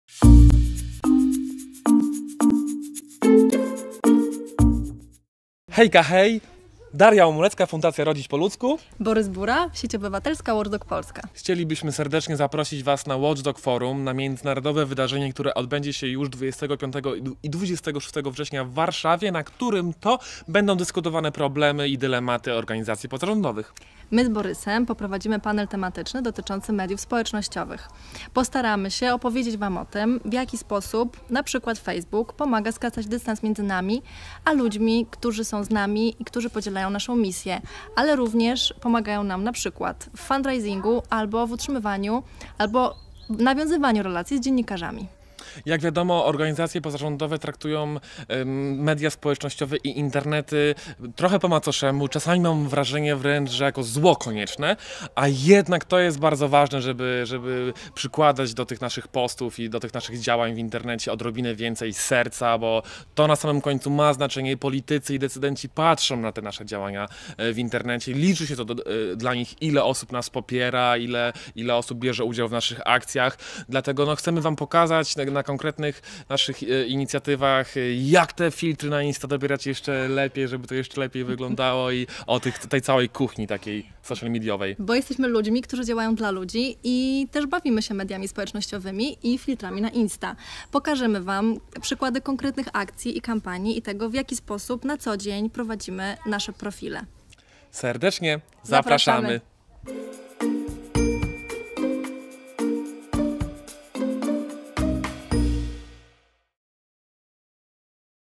Co siedzi w głowie watchdoga... Rola mediów społecznościowych w dialogu z ludźmi i decydentami
25-26 września w Warszawie odbędzie się Watchdog Forum. Będzie to niepowtarzalna okazja dla organizacji strażniczych do lepszego poznania się oraz porozmawiania o dylematach związanych z kontrolą obywatelską oraz narzędziach działania watchdogów. Podczas wydarzenia, w ramach stolików tematycznych, poruszonych zostanie 15 tematów odzwierciedlających aktualne problemy organizacji strażniczych.

W filmie Daria Omulecka z Fundacji Rodzić po Ludzku i Borys Bura z Sieci Obywatelskiej Watchdog Polska zapraszają do wspólnej dyskusji przy stoliku dotyczącym roli mediów społecznościowych w dialogu z ludźmi i decydentami. Pełen opis stolika oraz program Forum można znaleźć pod adresem